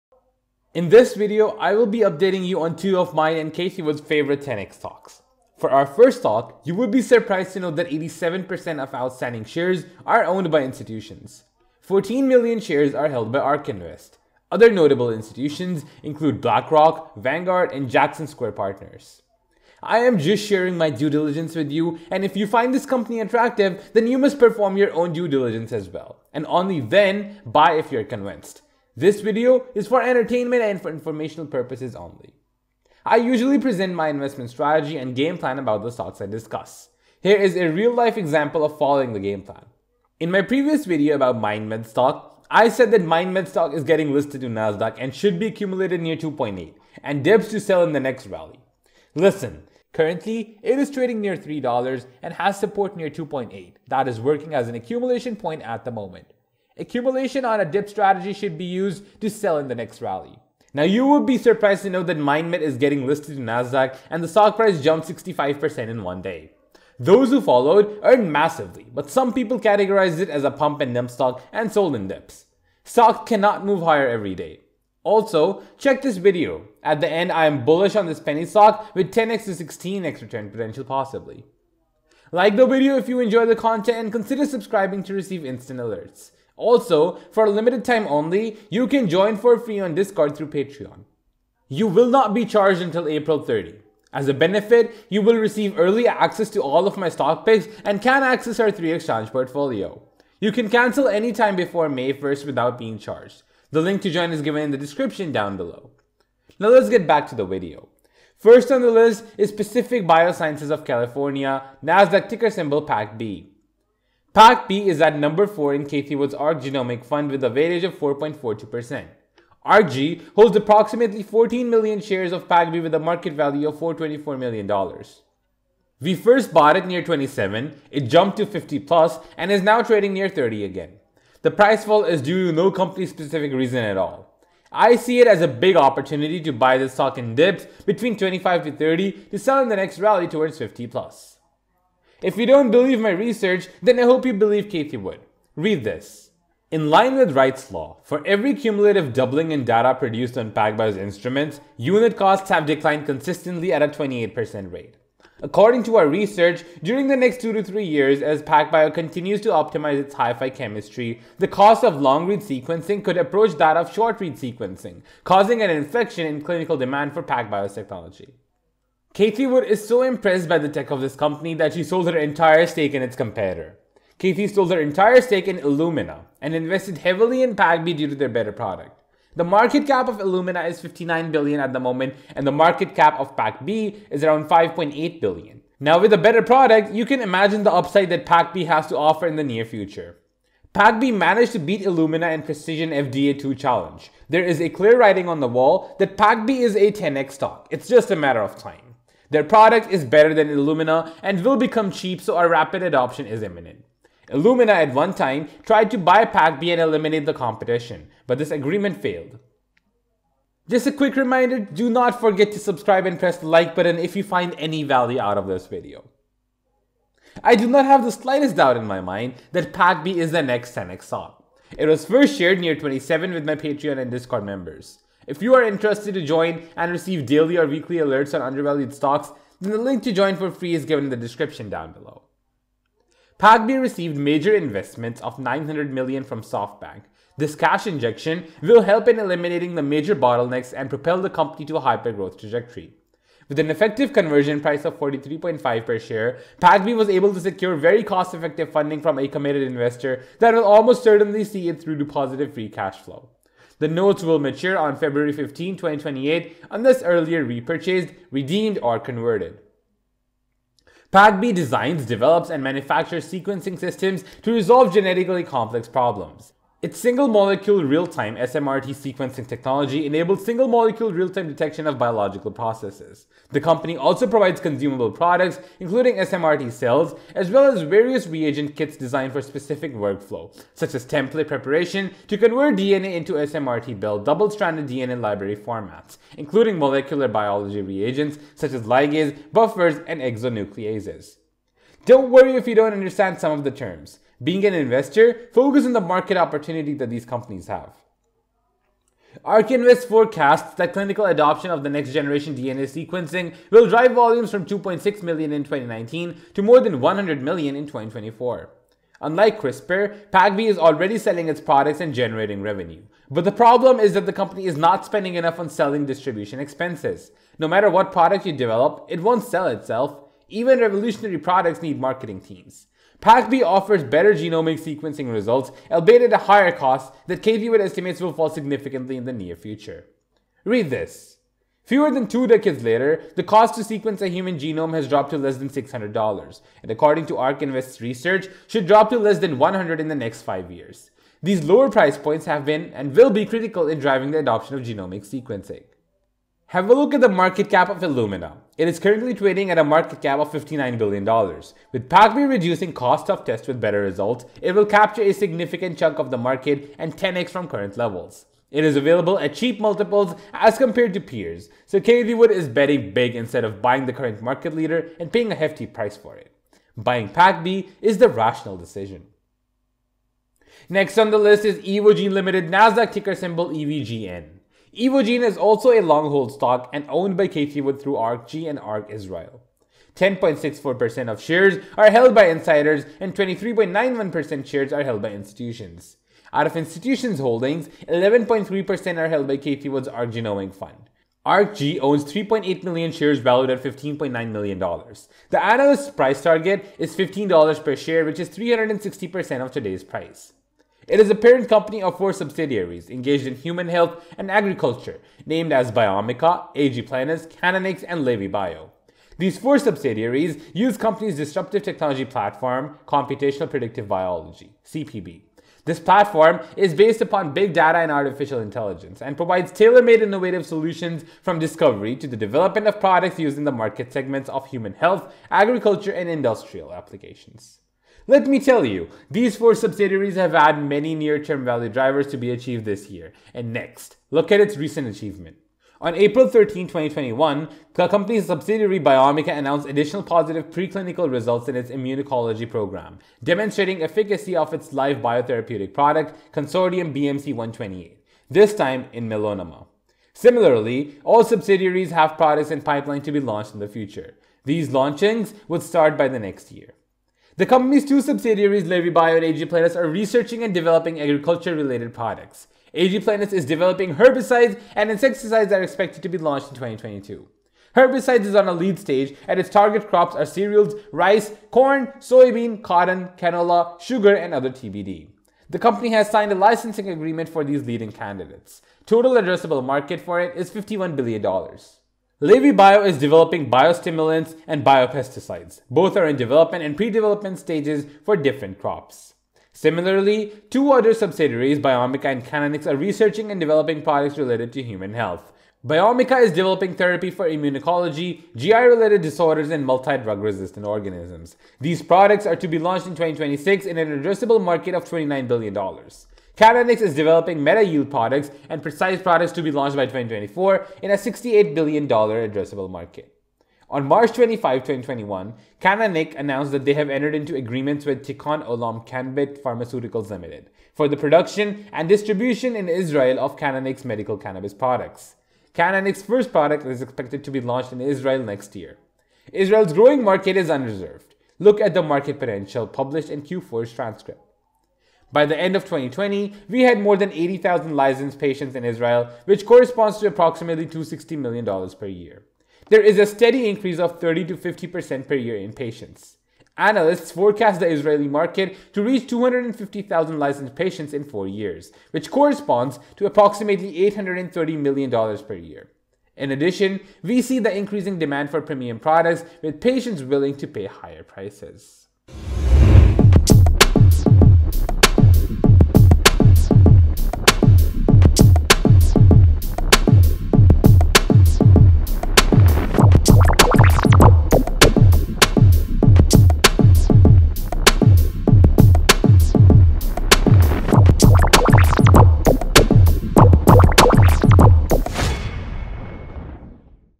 10x Stocks To Buy For Next Rally Cathie Wood Favorite Stocks
In this video I will be updating you on 2 of my and Cathie wood favourite 10x stocks. For our first stock you would be surprised to know that 87% of the outstanding shares are owned by institutions. 14 million shares are held by ark invest, other notable institutions include blackrock vanguard and Jackson square partners. I am just sharing my Due diligence with you and if you find this company attractive than you must perform your own due diligence and only than buy if you are convinced. This video is for entertainment and information purposes only.